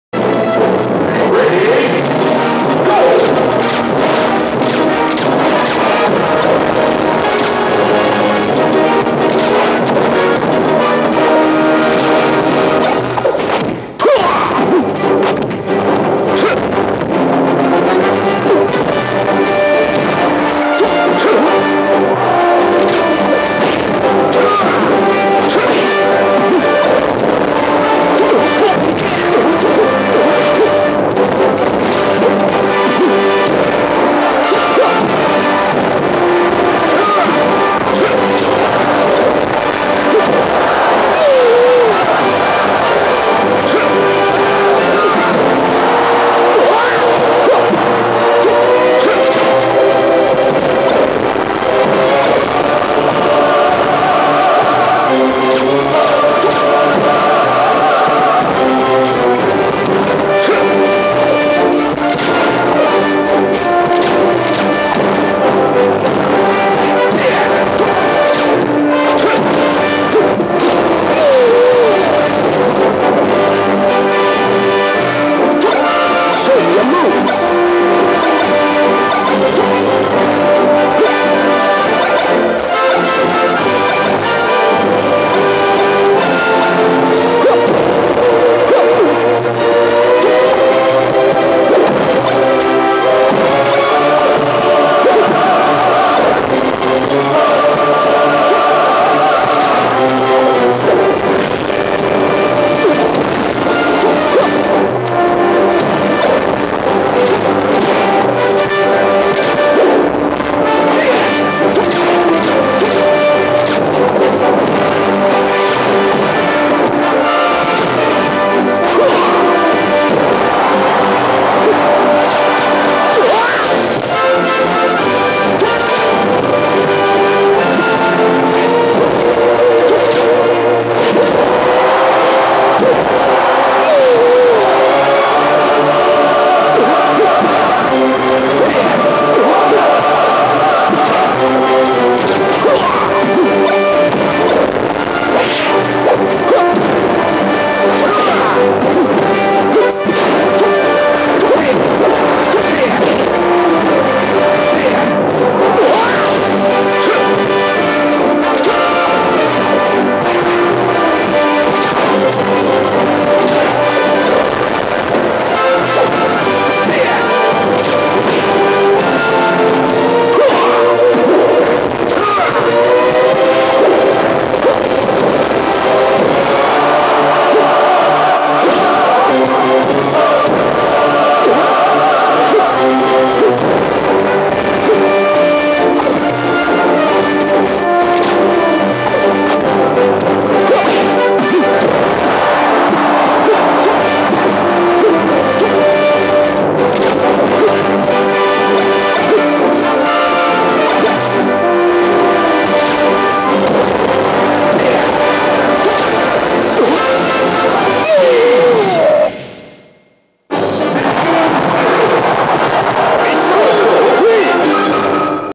FMF (Falco) vs AznDanny (Falcon) Round 2
10-7-06 FullMetalFalcomist (Falco) vs AznDanny (Falcon) Round 2
Pool friendlies at Spectre's